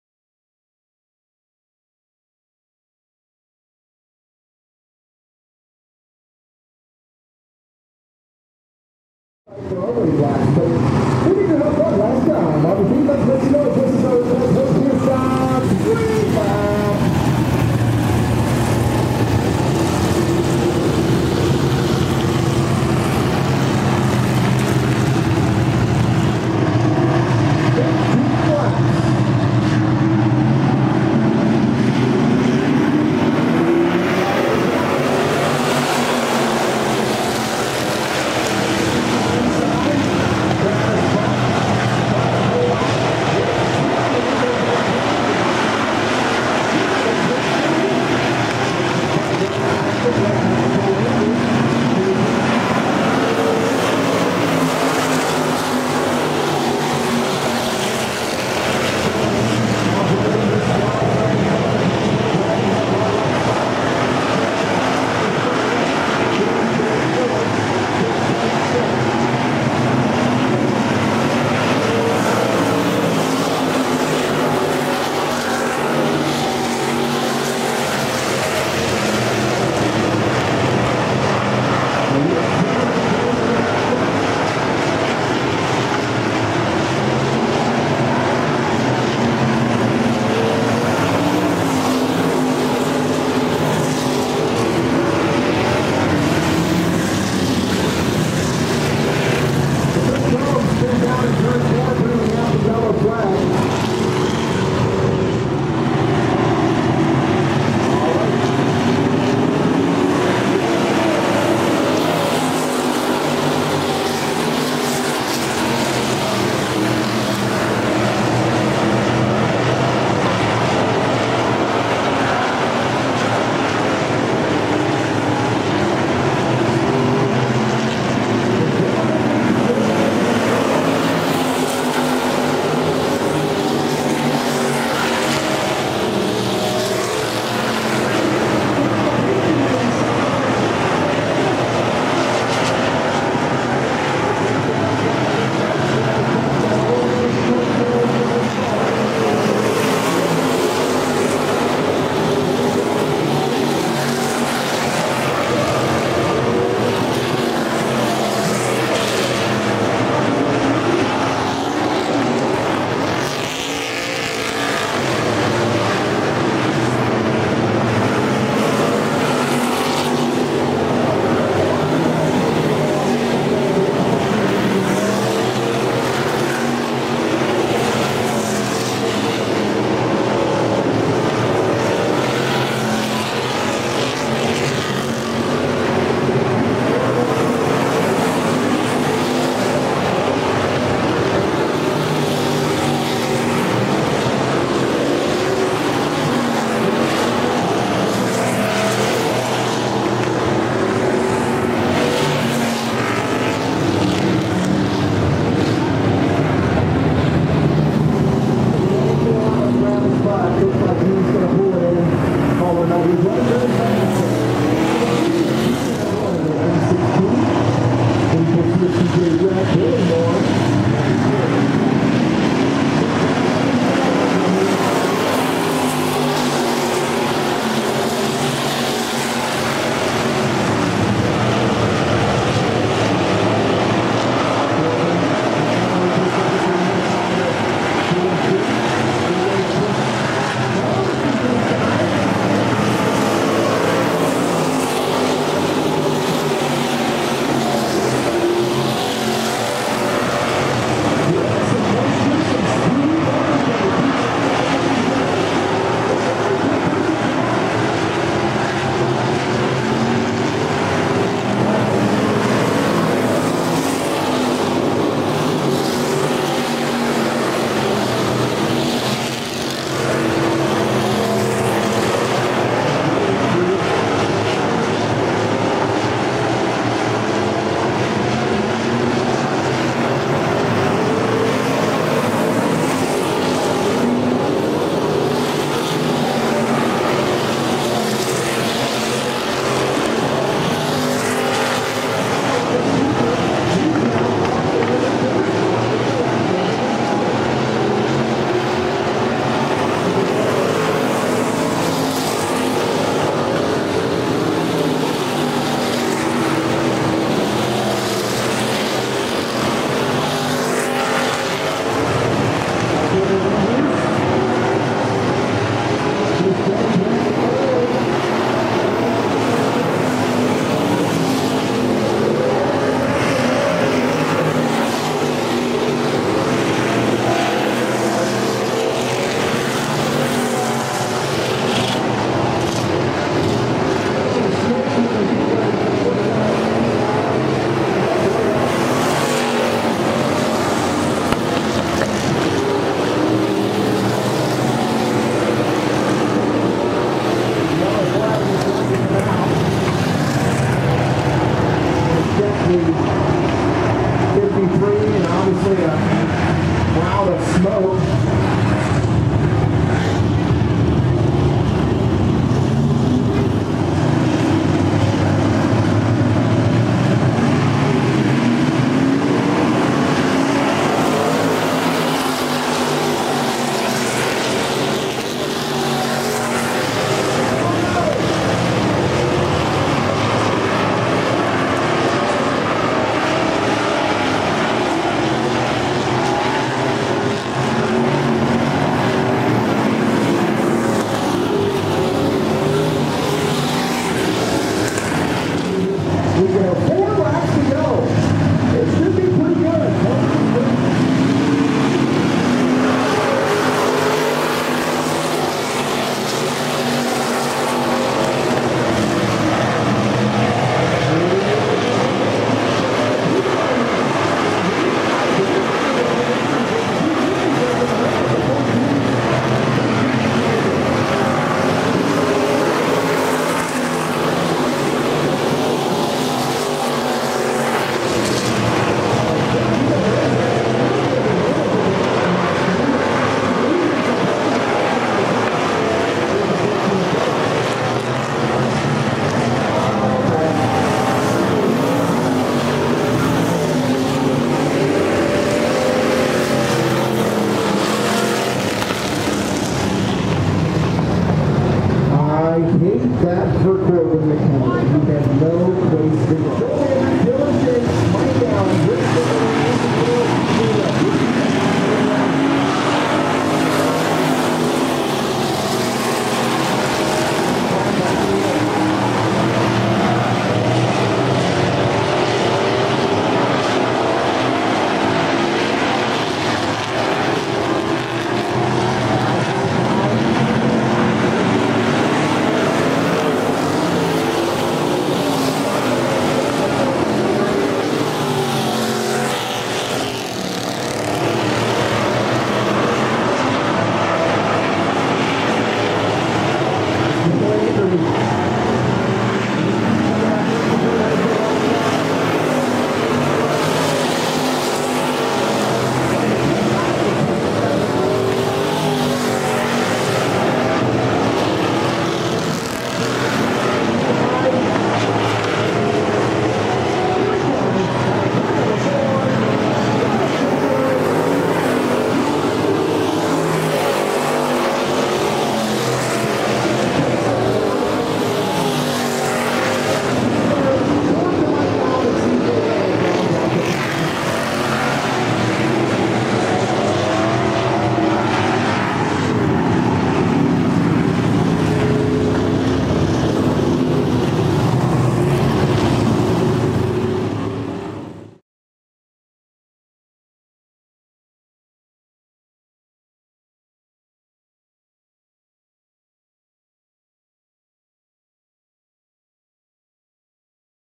Lawrenceburg Speedway Pure Stock Feature Race [8/5/23]
The A Main race for the Pure Stock class at Lawrenceburg Speedway on August 5th, 2023. 11 cars lined up for 15 pretty intense laps.

There was a good run for first, in the end, Joel Andrews (56) came away with the Victory! Congratulations!